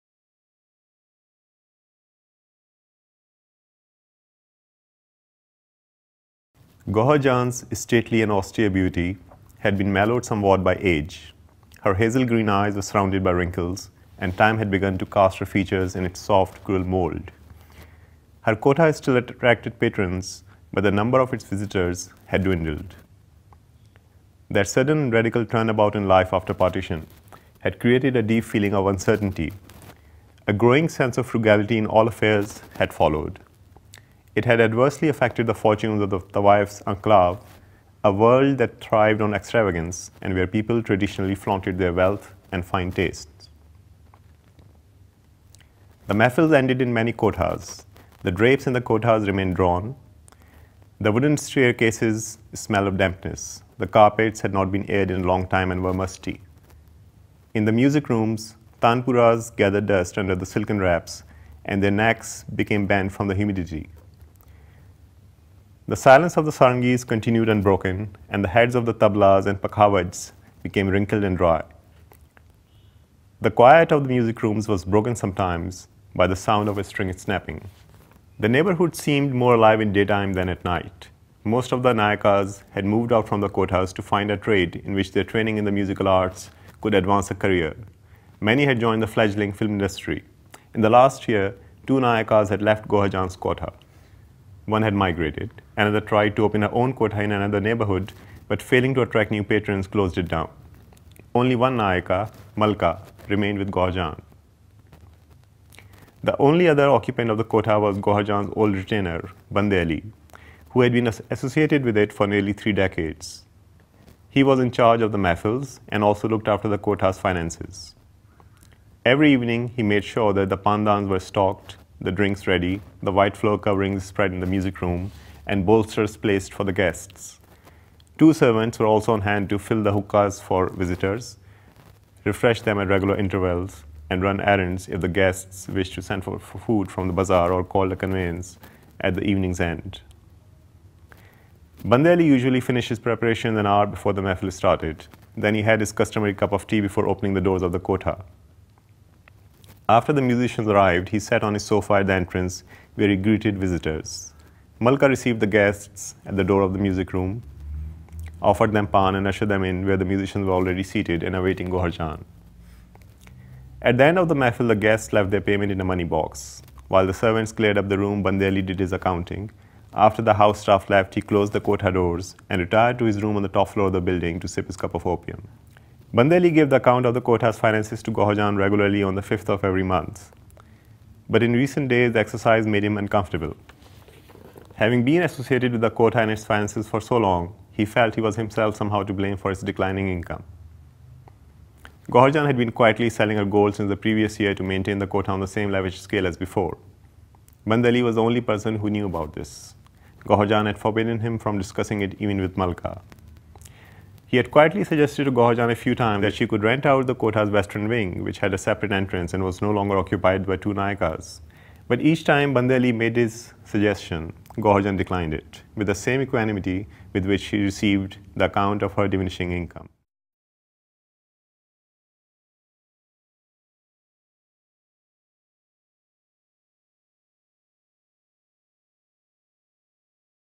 Musharraf Ali Farooqi reading from Between Clay and Dust
Through the lives of Ustad Ramzi, a champion wrestler and Gohar Jan, a famous courtesan, Musharraf Ali Farooqi tells a powerful story about love, honour, loss and redemption.